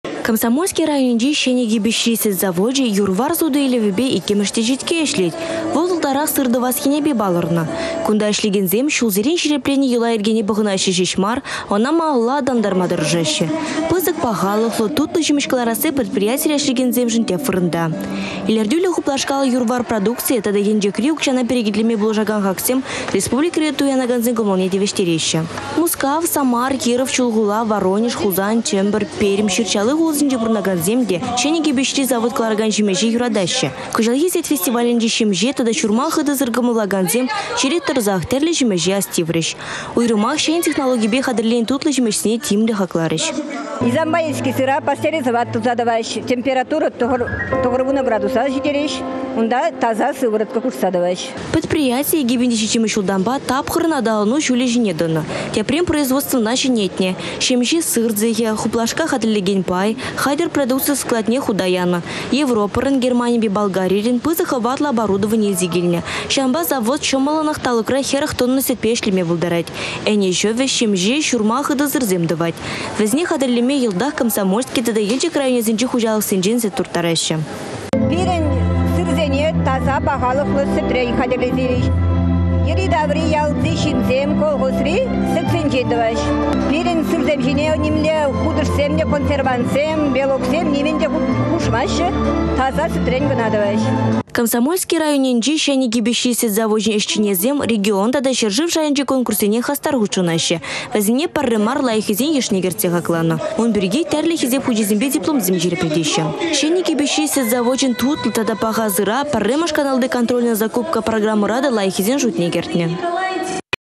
Çĕнĕ Кипеçри сĕт заводне çитекенни çук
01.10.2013
Комсомольски районĕнчи Çĕнĕ Кипеçри сĕт завочĕ юр-вар суту-илĕвĕпе  иккĕмĕш теçетке ĕçлет. Вăл ытларах сыр тăвас енĕпе палăрнă. Кунта ĕçлекенсем çулсерен çирĕпленнĕ йăла-йĕркене пăхăнаççĕ çеç мар, ăна малалла аталантарма тăрăшаççĕ.